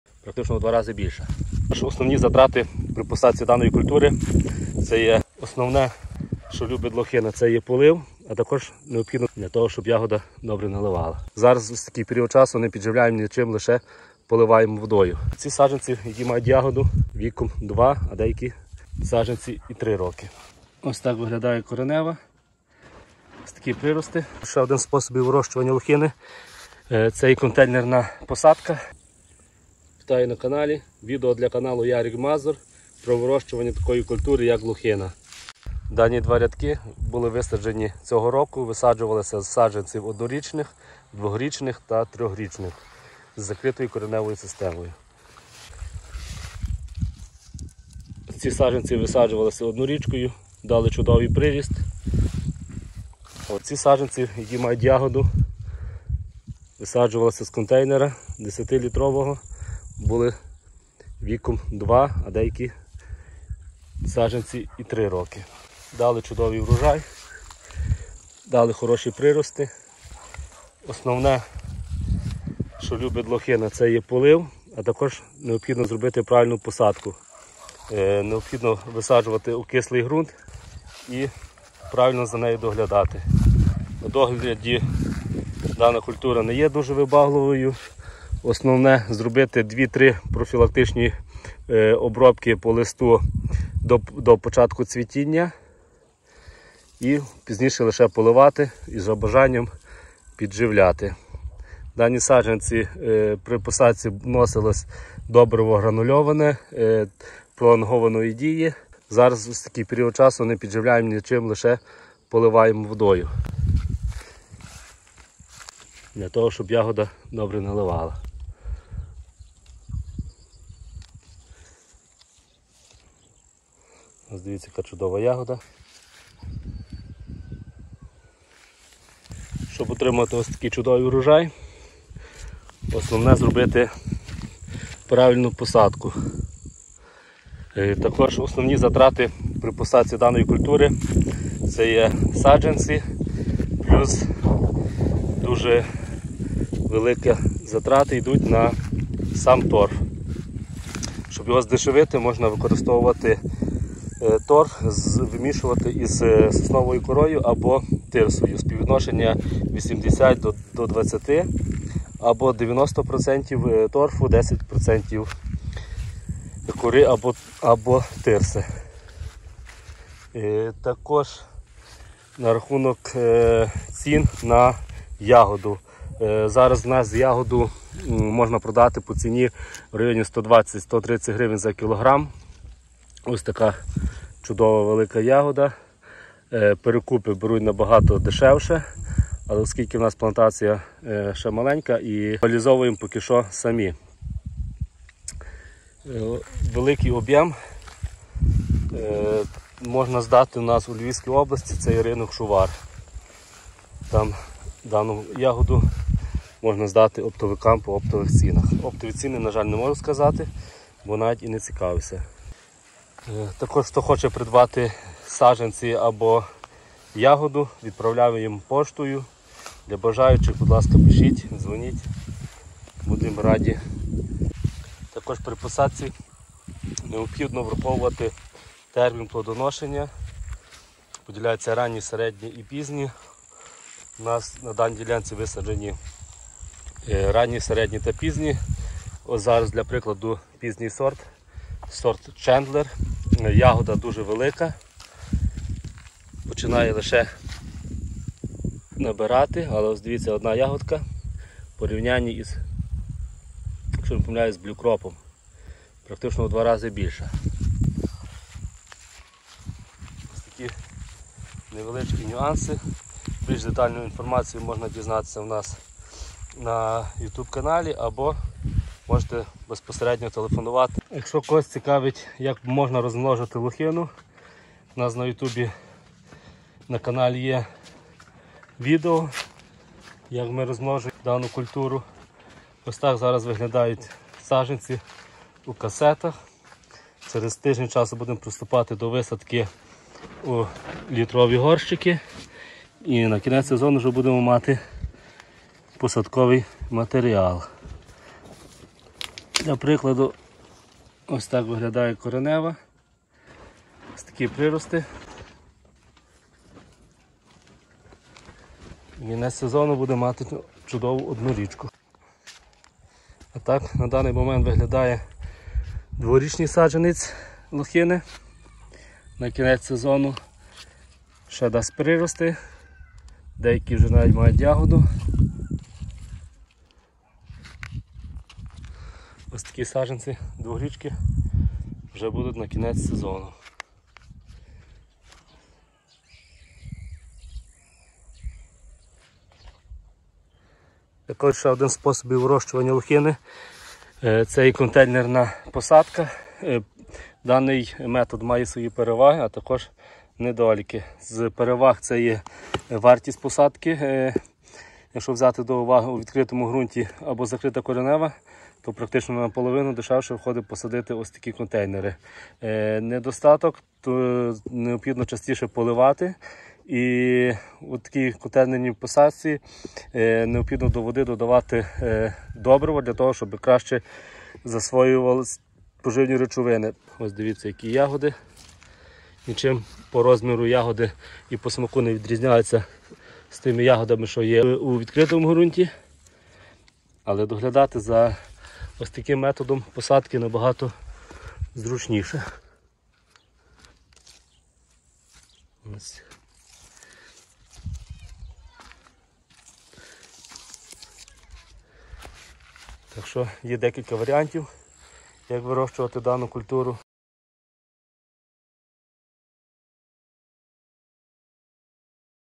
Лохина вирощування та догляд.‼️ Які саджанці і коли родять⁉️
Технологія вирощування Лохини від А до Я
Ярик Мазур